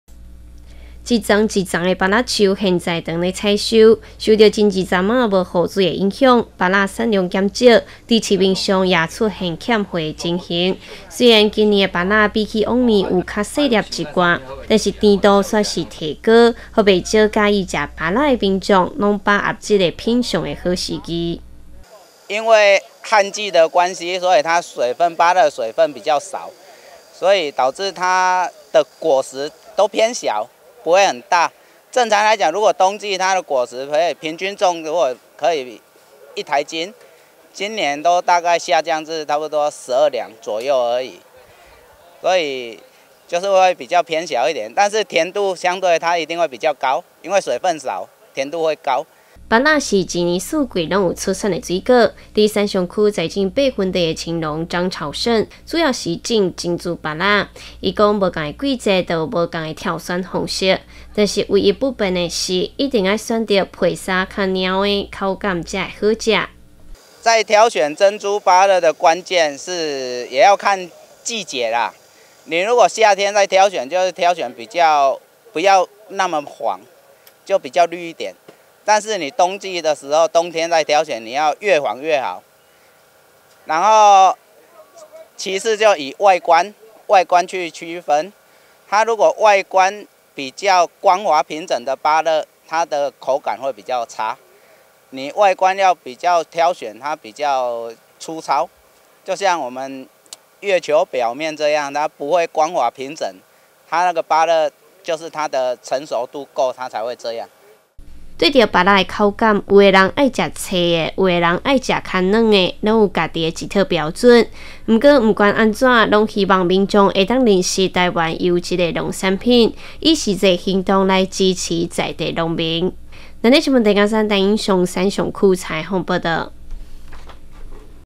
生長期遇旱季 芭樂果實小甜度卻更高
今年有不少農作物在生長期時受到缺水影響，果實長不大，像是芭樂也是一樣。在山上區栽種芭樂的果農張朝盛就表示，今年因為氣候的關係，所以芭樂的外觀比起往年是有較小一些，但是甜度卻更高。同時，他也順勢教大家要如何挑選好吃的芭樂，